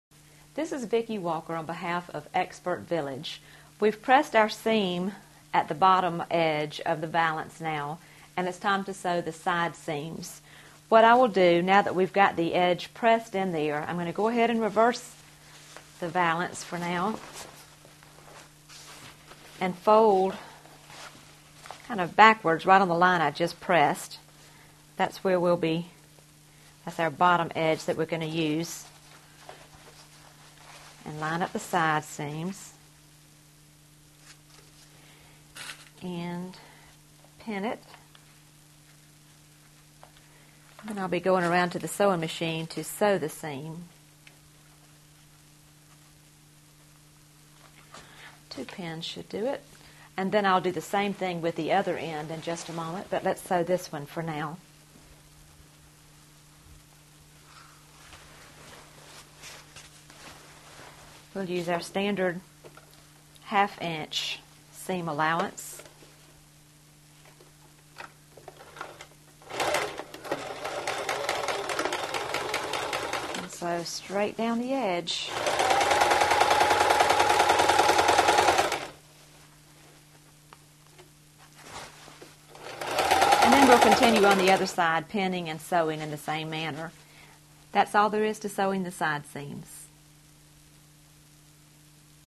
How To Pin & Sew Sides of Curtains
How To Pin & Sew Sides of Curtains. Part of the series: How to Sew Curtains. Learn how to pin and sew the sides of the curtains for a curtain sewing project with expert sewing instructions in this free crafting video clip.